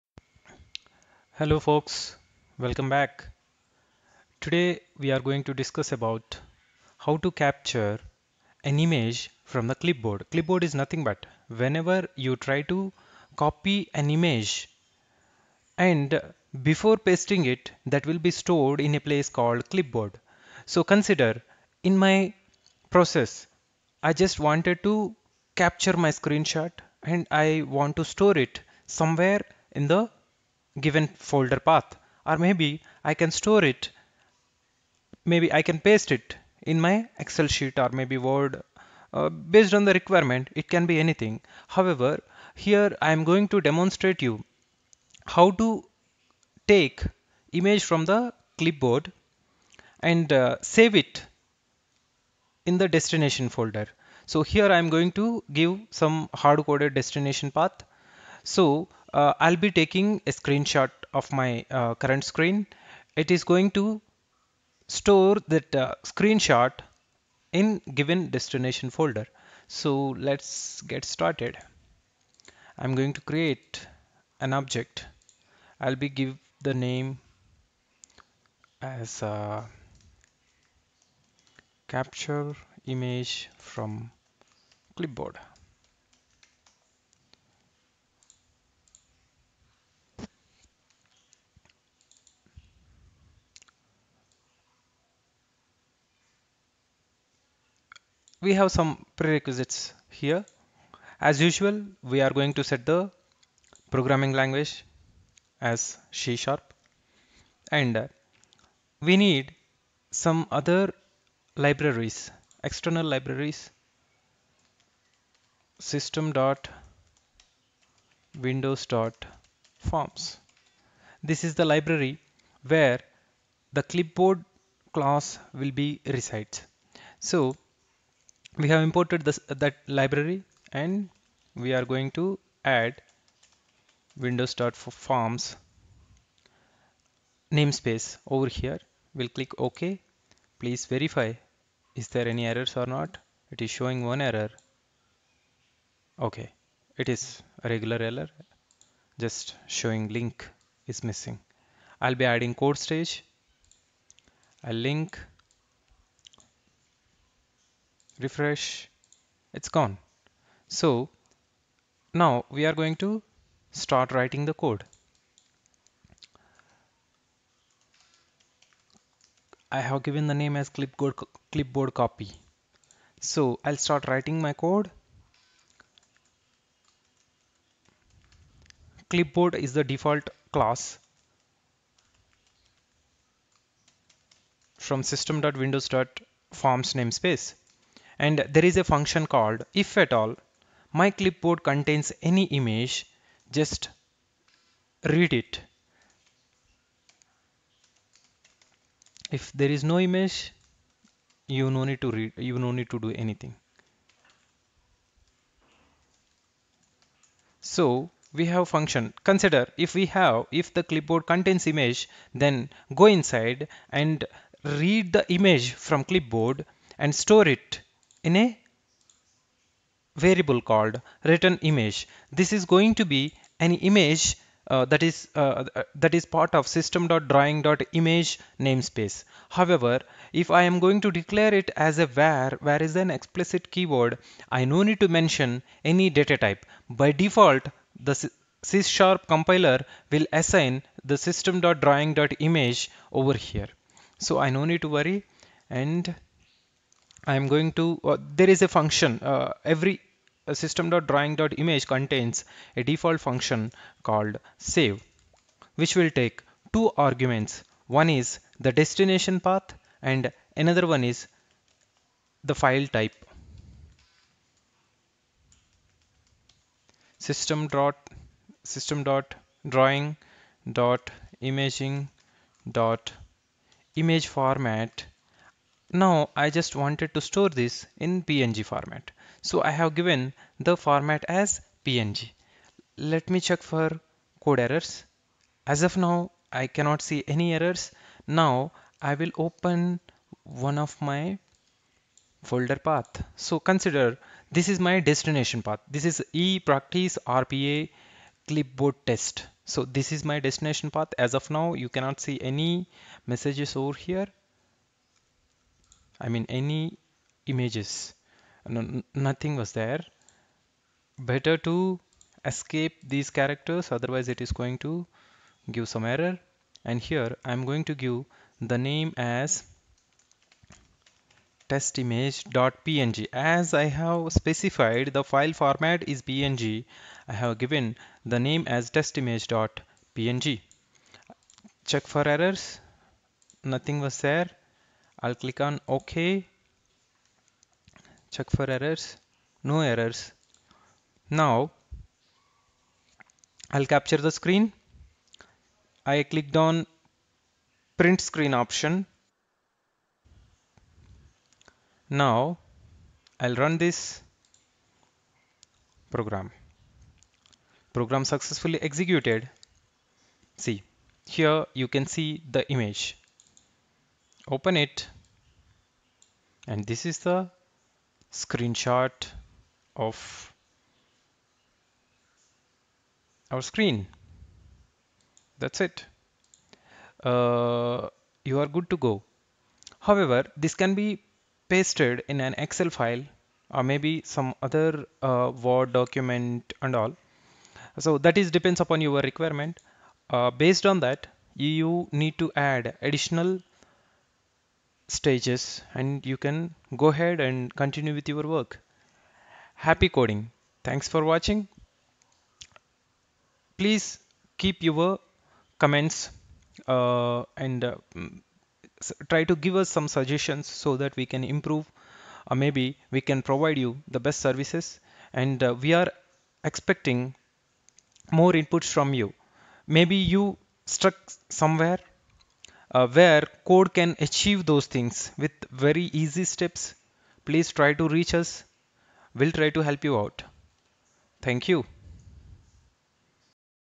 Blue Prism Code Stage Read Image from Clipboard
Read Image from Clipboard with Code Stage, Store it in folder path.